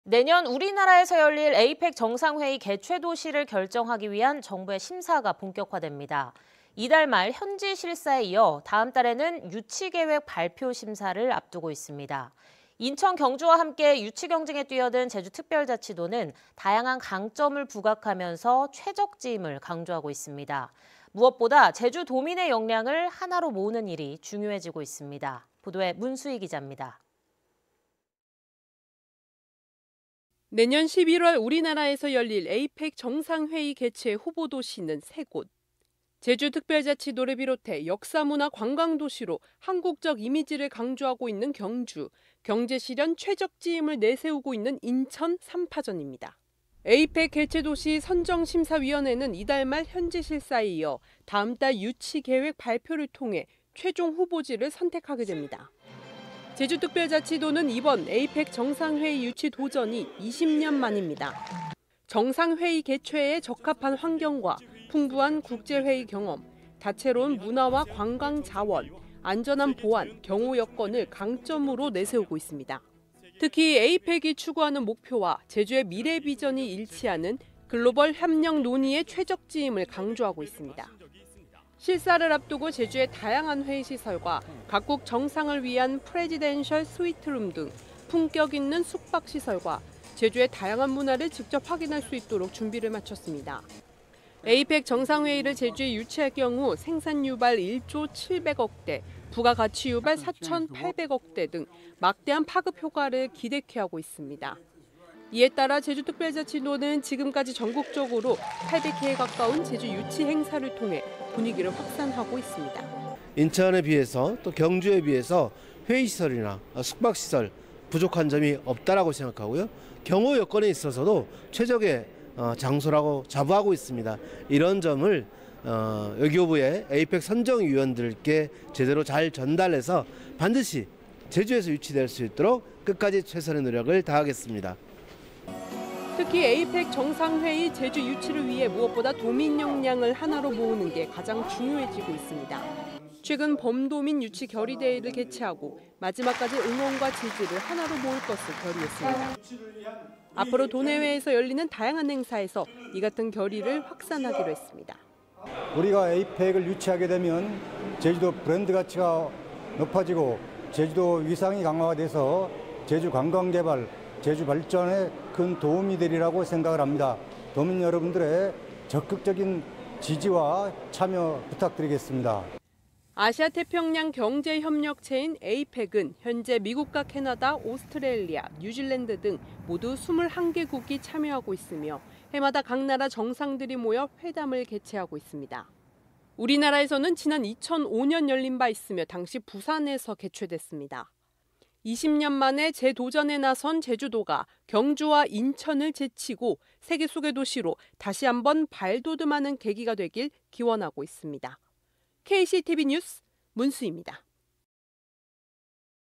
APEC 개최도시 이달부터 심사…"제주 최적지"
내년 우리나라에서 열릴 APEC 정상회의 개최도시를 결정하기 위한 정부의 심사가 본격화됩니다.

이달 말 현지 실사에 이어 다음달에는 유치계획 발표 심사를 앞두고 있습니다.

인천, 경주와 함께 유치경쟁에 뛰어든 제주특별자치도는 다양한 강점을 부각하면서 최적지임을 강조하고 있습니다.

무엇보다 제주도민의 역량을 하나로 모으는 일이 중요해지고 있습니다.

보도에 문수희 기잡니다.

내년 11월 우리나라에서 열릴 APEC 정상회의 개최 후보도시는 3곳.

제주특별자치도를 비롯해 역사문화관광도시로 한국적 이미지를 강조하고 있는 경주, 경제실현 최적지임을 내세우고 있는 인천간 3파전입니다.

APEC 개최도시 선정심사위원회는 이달 말 현지실사에 이어 다음달 유치계획 발표를 통해 최종 후보지를 선택하게 됩니다.

제주특별자치도는 이번 APEC 정상회의 유치 도전이 20년만입니다.

정상회의 개최에 적합한 환경과 풍부한 국제회의 경험, 다채로운 문화와 관광자원, 안전한 보안.경호 여건을 강점으로 내세우고 있습니다.

특히 APEC이 추구하는 목표와 제주의 미래비전이 일치하는 글로벌 협력 논의의 최적지임을 강조하고 있습니다.

실사를 앞두고 제주의 다양한 회의시설과 각국 정상을 위한 프레지덴셜 스위트룸 등 품격 있는 숙박시설과 제주의 다양한 문화를 직접 확인할 수 있도록 준비를 마쳤습니다.

APEC 정상회의를 제주에 유치할 경우 생산유발 1조 700억대, 부가가치유발 4천 800억대 등 막대한 파급효과를 기대케 하고 있습니다.

이에따라 제주특별자치도는 지금까지 전국적으로 800회에 가까운 제주 유치 행사를 통해 분위기를 확산하고 있습니다.

[오영훈 / 제주특별자치도지사]
"인천과 경주에 비해 회의, 숙박시설 부족한 점이 없다고 생각하고 경호 여건에 있어서도 최적의 장소라고 자부하고 있습니다. 이런 점을 외교부에, APEC선정 위원들에게 잘 전달해서 반드시 제주에서 유치될 수 있도록 끝까지 최선을 노력을 다하겠습니다."

특히 APEC 정상회의의 제주 유치를 위해 무엇보다 도민 역량을 하나로 모으는게 가장 중요해지고 있습니다.

최근 범도민 유치 결의대회를 개최하고 마지막까지 응원과 지지를 하나로 모을 것을 결의했습니다.

앞으로 도내외에서 열리는 다양한 행사에서 이같은 결의를 확산하기로 했습니다.

[양문석 / APEC 제주유치 범도민추진위 공동위원장]
"APEC을 유치하게 되면 제주의 브랜드 가치가 높아지고 제주도의 위상이 강화돼서 제주 관광개발, 발전에 큰 도움이 될 것이라고 생각합니다. 도민 여러분들의 적극적인 지지와 참여 부탁합니다."

아시아태평양경제협력체인 APEC은 현재 미국과 캐나다, 오스트레일리아, 뉴질랜드 등 21개국이 참여하고 있으며 해마다 각 나라의 정상들이 모여 회담을 개최하고 있습니다.

우리나라에서는 지난 2005년 열린 바 있으며 당시 부산에서 개최됐습니다.

20년만에 재도전에 나선 제주특별자치도가 경주와 인천을 제치고 세계속의 도시로 다시 한번 발돋음하는 계기가 되기를 기원하고 있습니다.

KCTV뉴스 문수희입니다.

(영상취재 : 현광훈)